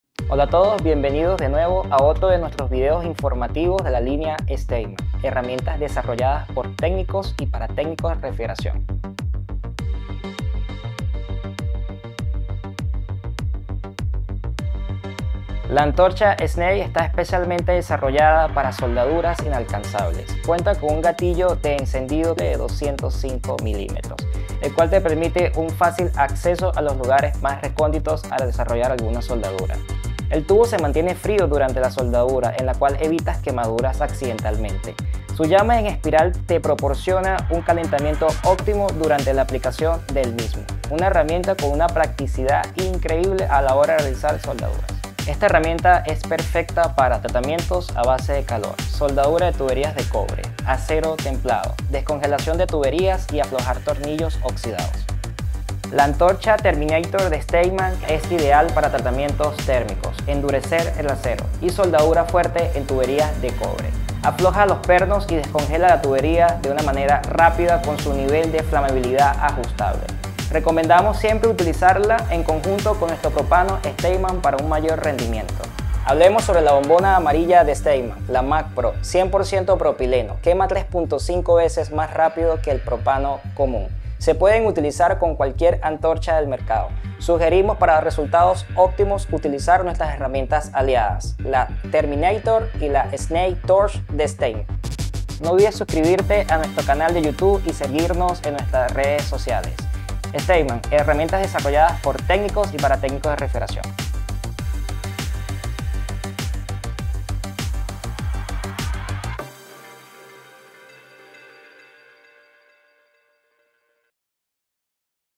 STEINMANN - Antorchas
En este tercer video, te mostraremos las características esenciales de nuestras antorchas Steinmann.

Como siempre lo mencionamos, nuestro compromiso es brindarte las mejores herramientas para hacer tu trabajo más sencillo y efectivo.

Steinmann!

¡Y, por supuesto, déjanos tus comentarios!